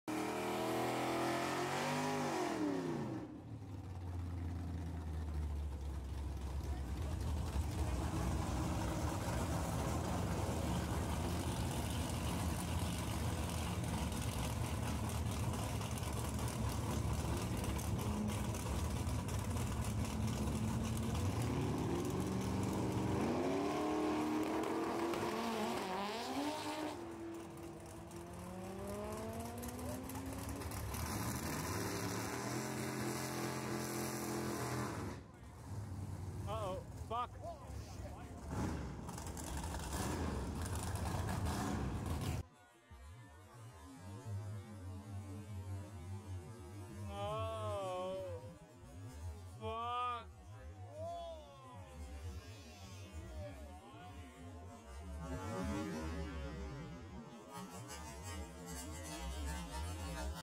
AllTrac breaks after misshift.
A rough day at the dragstrip for BADNEWS. Seems the engine doesn't appreciate 14,000 RPM.